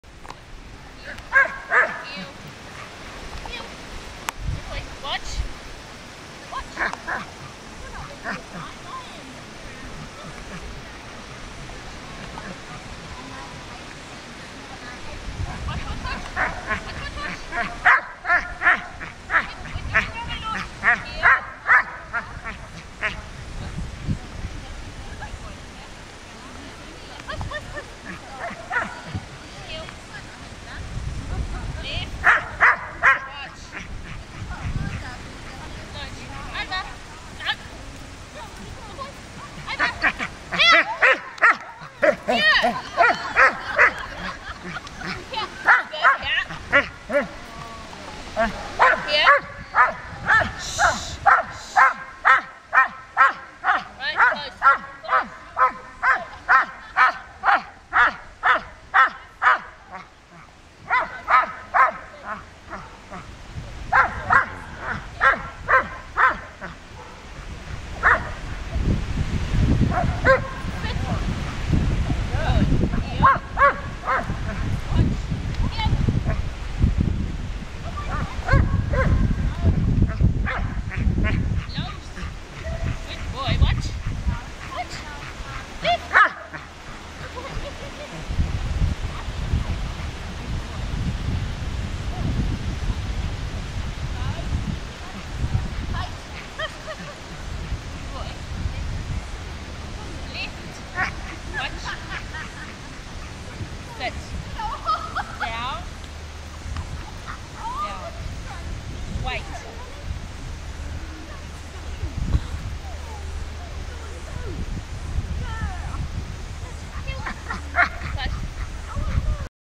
rally advanced training
pretty funny!!! highlights for me Launch reading the first couple of signs, and the double jumps, Launch had soooo much fun. (me too)